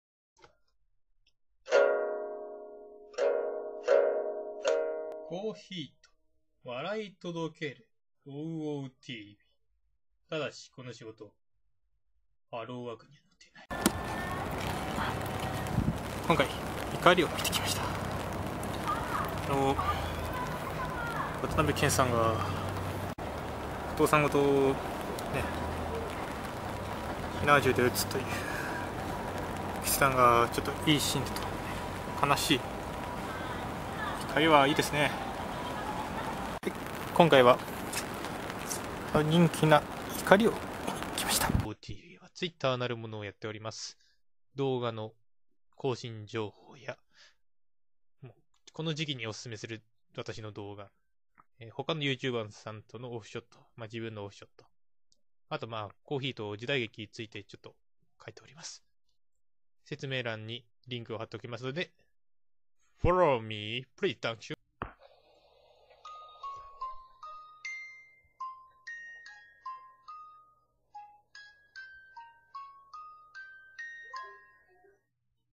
話題の映画怒りを０円で見てきたのだ
映画怒りを見てきたぞ横浜には０円で怒りを見れるところがあるんだ
千葉編の渡辺謙さん、宮崎あおいさん、松山ケンイチさん、沖縄編の広瀬すずさん、森山未来さん、東京編の妻夫木聡さん、綾野剛さん